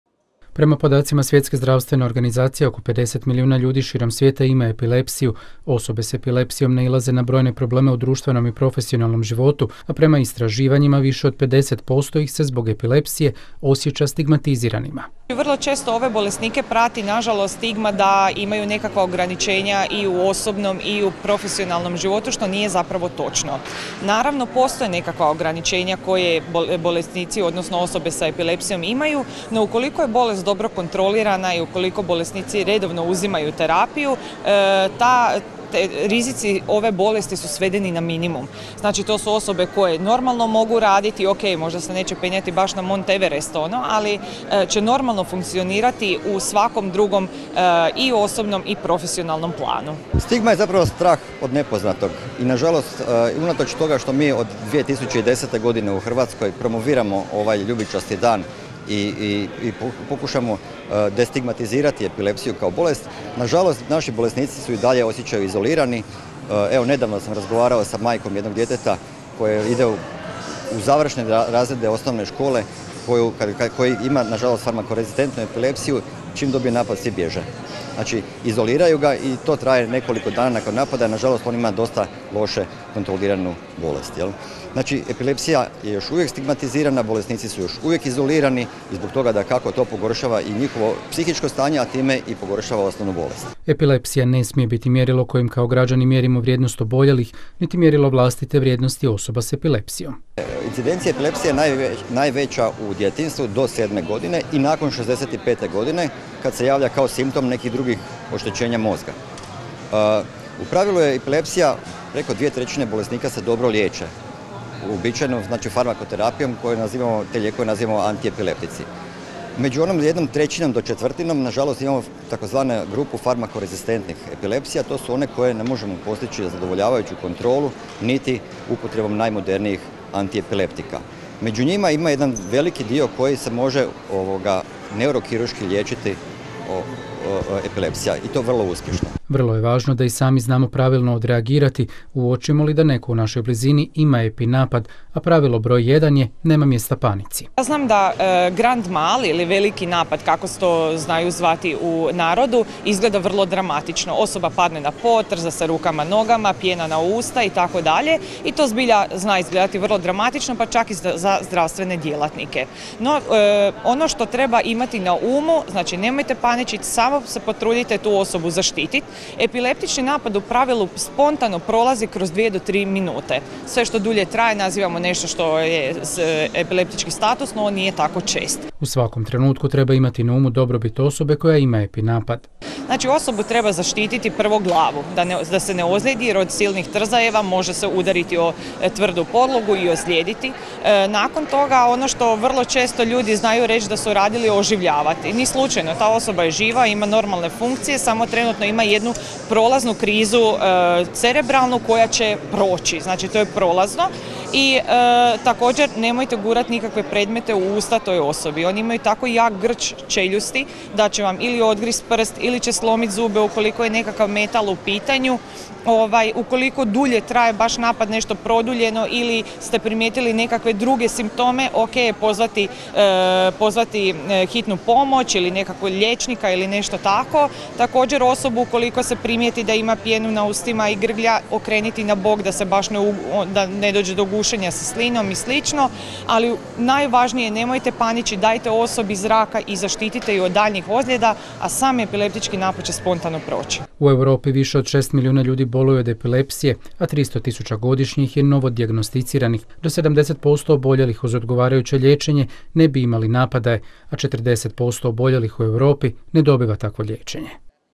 U Osijeku je obilježen Ljubičasti dan kao podrška osobama s epilepsijom
U Osijeku je obilježen Ljubičasti dan kao podrška osobama s epilepsijom. Purple day se obilježava na svim kontinentima i u više od 100 zemalja svijeta te je proglašen najutjecajnijim međunarodnim danom u svrhu podizanja razine osviještenosti o epilepsiji. U Europi više od 6 milijuna ljudi boluje od epilepsije, a svake godine dijagnosticira se 300,000 novih slučajeva. Gotovo 70% oboljelih uz odgovarajuće liječenje ne bi imalo napadaje, no 40% oboljelih u Europi ne dobiva takvo liječenje.